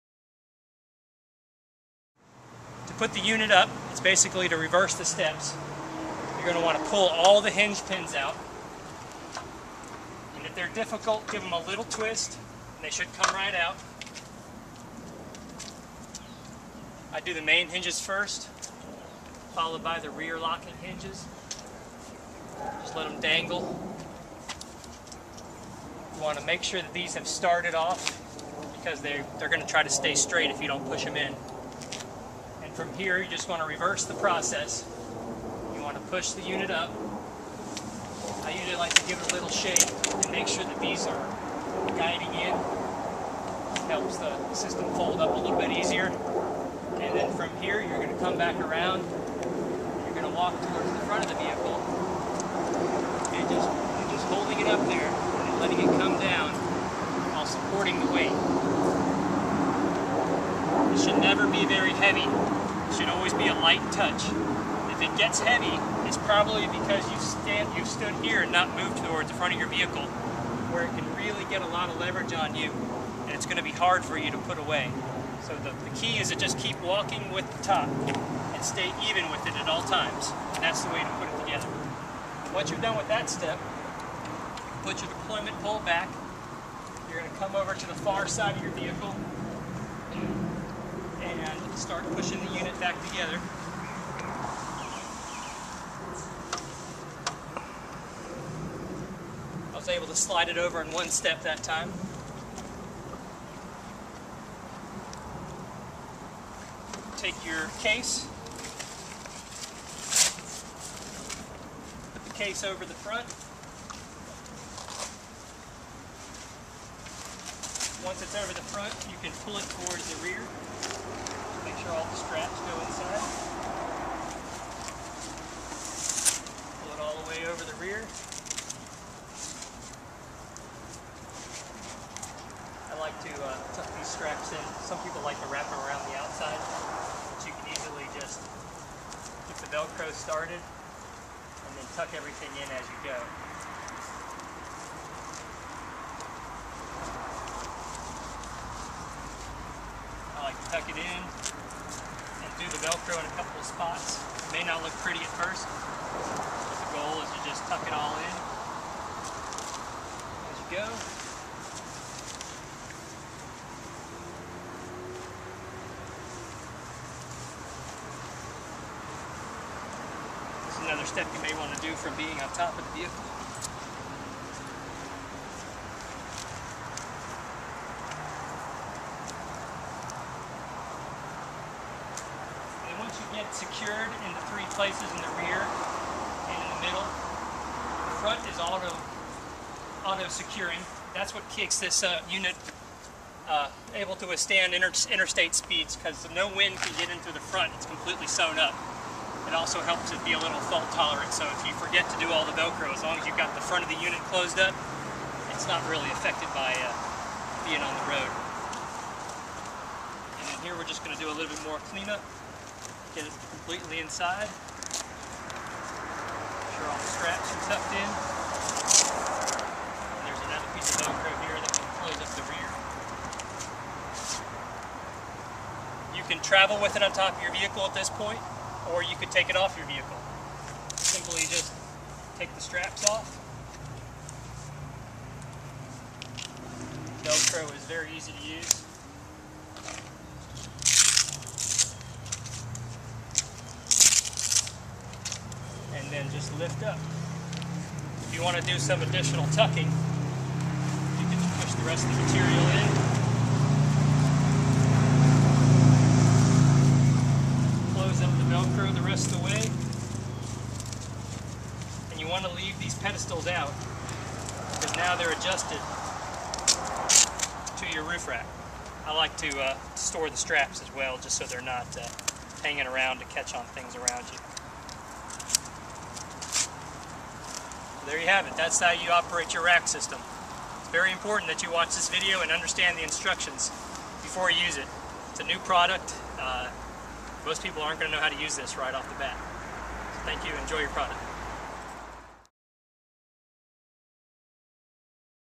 RACC Take-down Part 2
I know you will want to start using your RACC system ASAP, but it is important to note that this is a brand new invention and you WILL NOT know how to use it unless you read the instructions or someone demonstrates it for you.  Below you will find two comprehensive videos to show you how to setup and take-down your new RACC system, starring me, the Inventor/Engineer Paul Jankowski.  The RACC System is very easy to use, if you find yourself struggling or not strong enough, you are not using the correct technique.  The RACC frame is designed to give you many years of reliable operation if used according to these instructions.